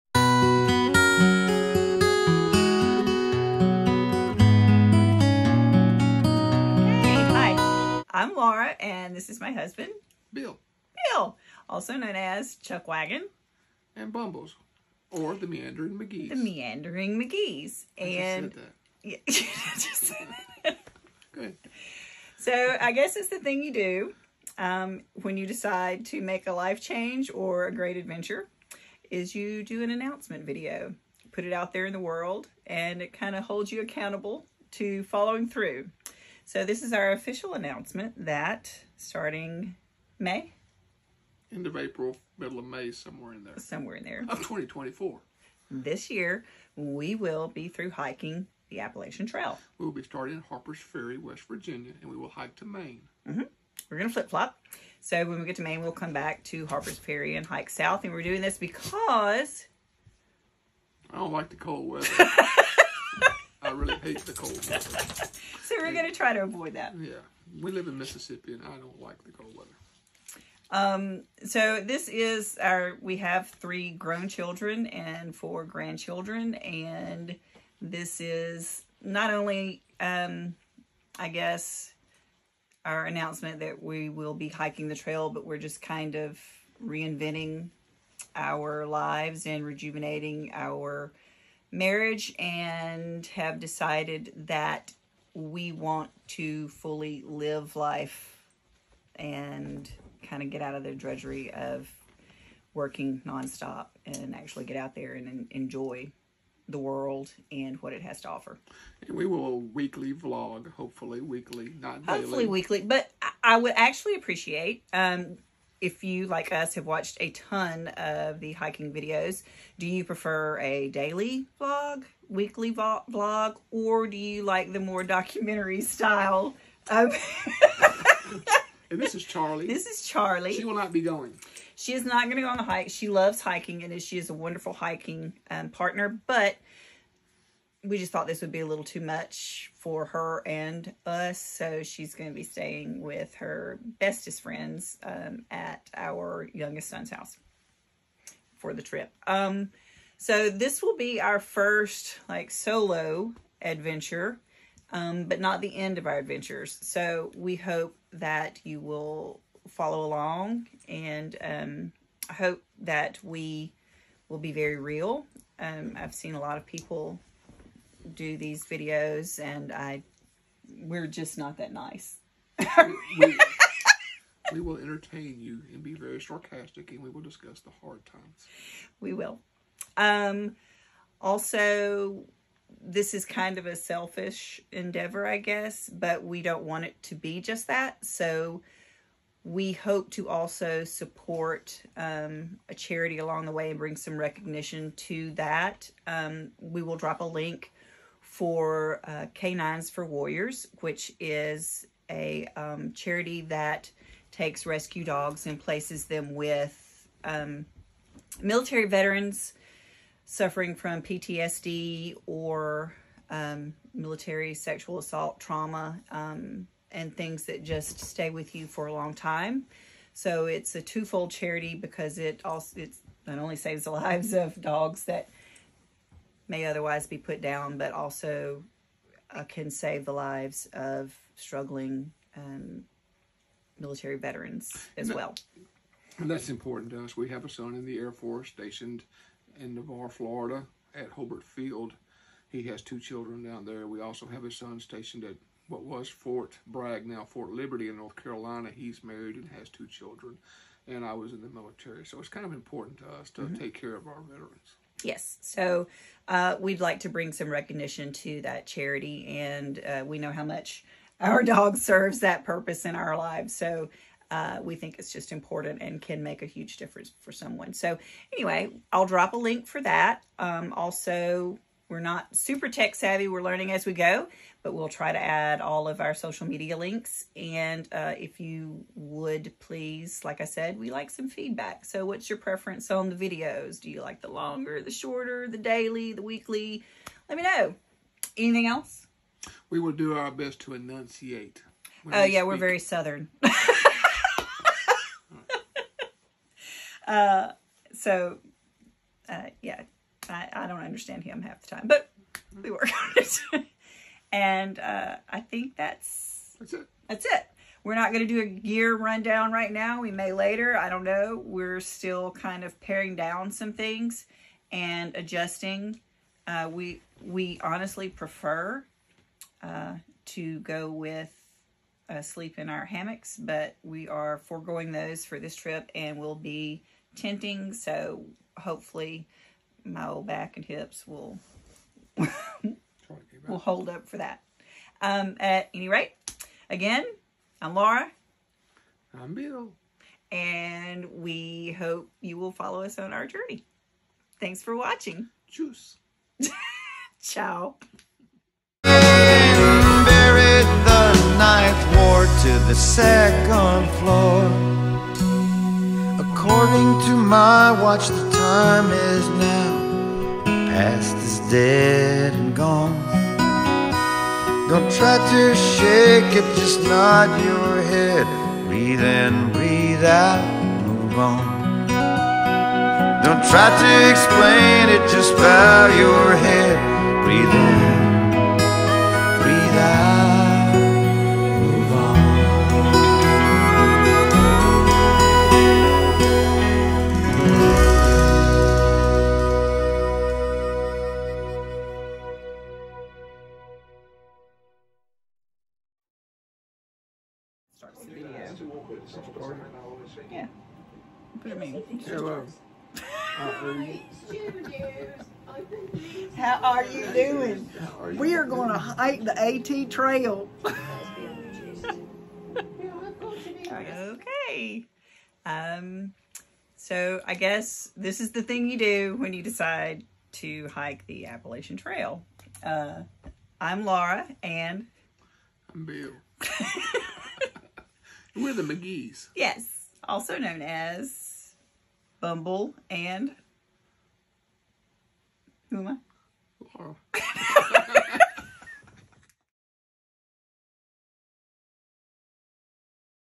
We’re doing it! 2024 Appalachian Trail thru hike (FlipFlop style)
It’s time for a directional change in our lives. First on the adventure list is hiking the Appalachian Trail (2024)! We hope you will join us on this adventure and follow our journey into the next chapter of our lives.